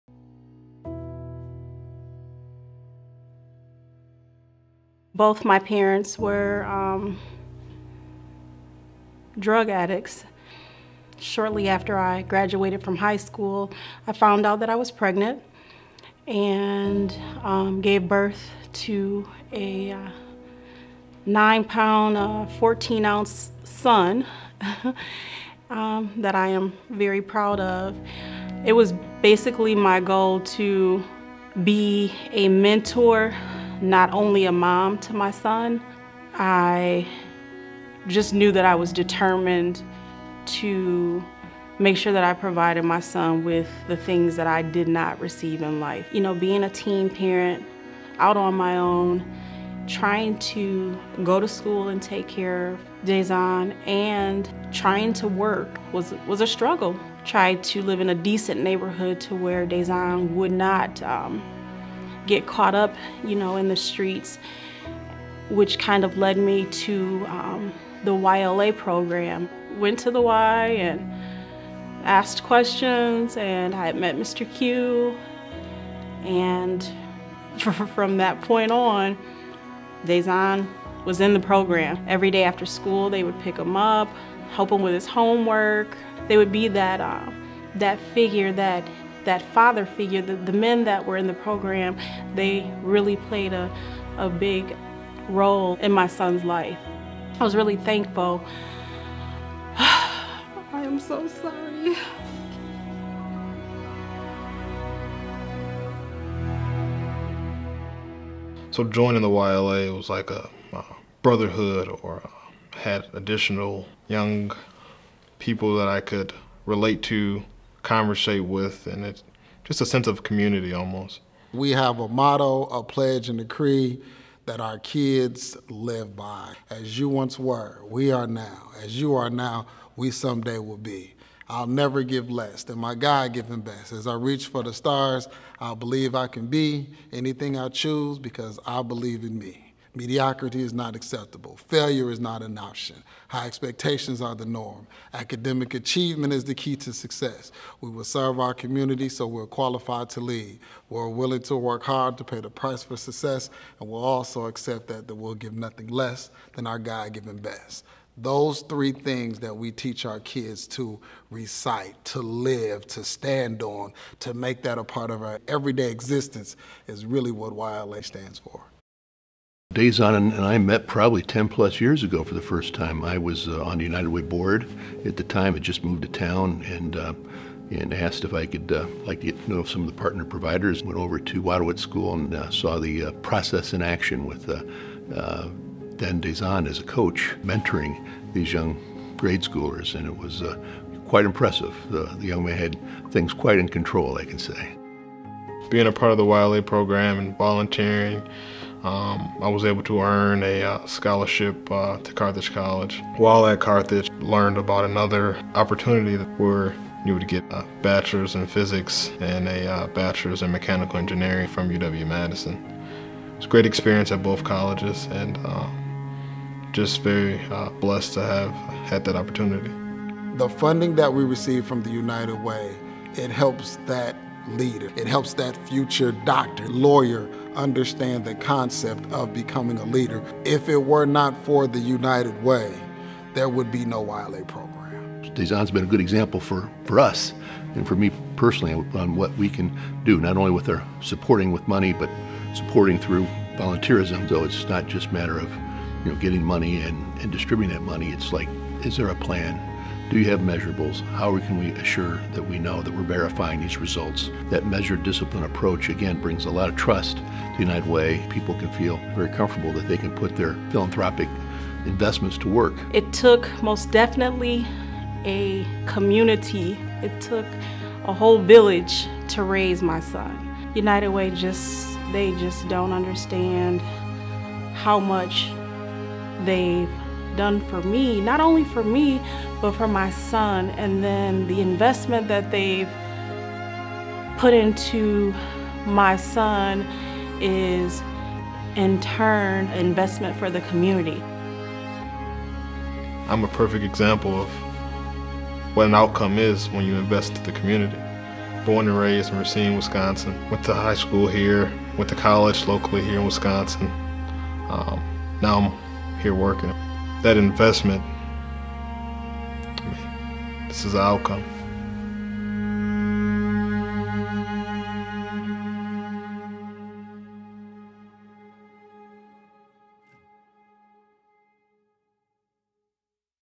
The Outcome of Your Investment | 2019 United Way of Racine County Campaign Video
United Way of Racine County fights for the health, education, and financial stability of every person in our community. DaZahn's success is just one example of what we can achieve as a community when we LIVE UNITED.